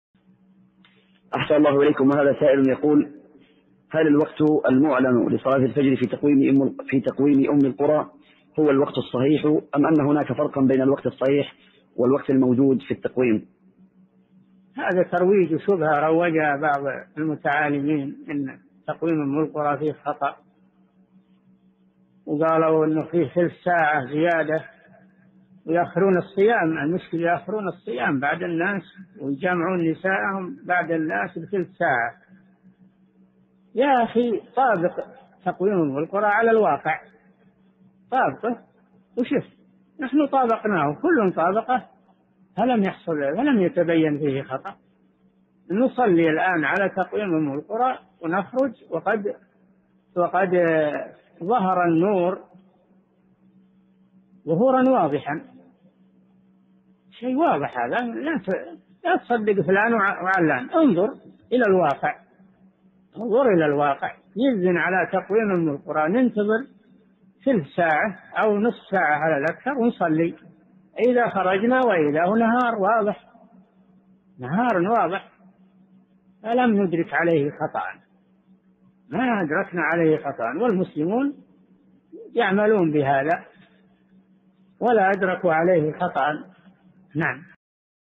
[54 /186] هل الوقت المعلن لصلاة الفجر في تقويم [أم القرى] هو الوقت الصحيح،... | الشيخ صالح الفوزان
[54 /186] هل الوقت المعلن لصلاة الفجر في تقويم [أم القرى] هو الوقت الصحيح، أم أن هناك فرقا بين الوقت الصحيح والوقت الموجود في التقويم؟
الجواب: هذا ترويج وشبهة روجها بعض المتعالمين أن تقويم [أم القرى] فيه خطأ، وقالوا أن فيه ثلث ساعة زيادة ويؤخرون الصيام، المشكلة يؤخرون الصيام بعد الناس ويجامعون نساءهم بعد الناس بثلث ساعة، يا أخي طابق تقويم أم القرى على الواقع طابقه وشفت، نحن طبقناه وكل طابقه فلم يتبين فيه خطأ، نصلي الآن على تقويم [أم القرى] ونخرج وقد ظهر النور ظهورًا واضحًا، شيء واضح هذا، لا تصدق فلان وعلان، انظر إلى الواقع، يؤذن على تقويم [أم القرى] ننتظر ثلث ساعة أو نصف ساعة على الأكثر ونصلي فإذا خرجنا وإذا هو نهار واضح، فلم ندرك عليه خطأ، ما أدركنا عليه خطأ والمسلمون يعملون بهذا ولا أدركوا عليه خطأ.
السيرة الذاتية :
للاشتراك في قناة الشيخ صالح الفوزان

للاستفسار والإقتراحات والدعم
رئيس الفريق
لمشروع كبار العلماء